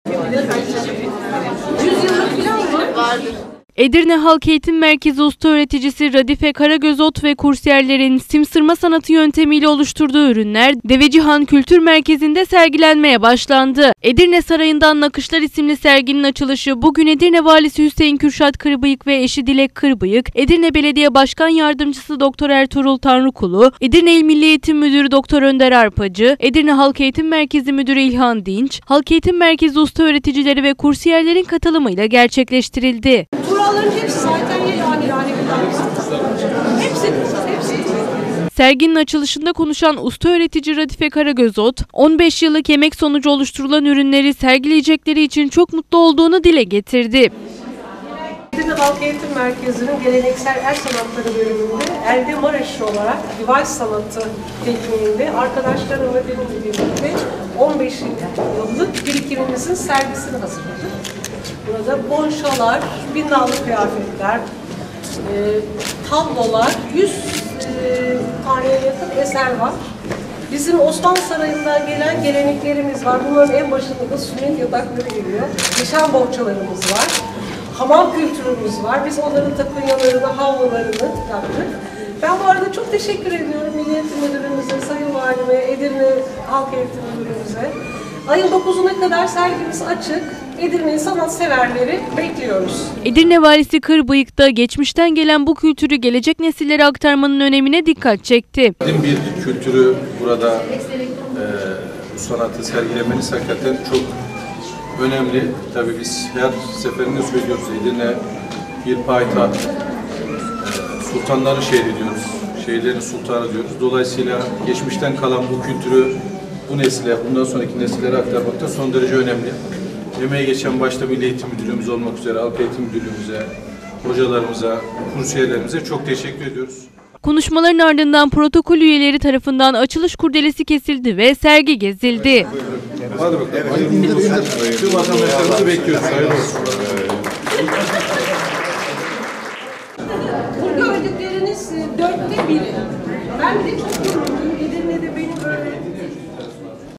‘EDİRNE SARAYI’NDAN NAKIŞLAR’ SERGİLENDİ
Edirne Halk Eğitim Merkezi’nde devam eden Sim Sırma Kursunda yapılan 100’e yakın ürün, Devecihan Kültür Merkezi’nde sergilenmeye başlandı.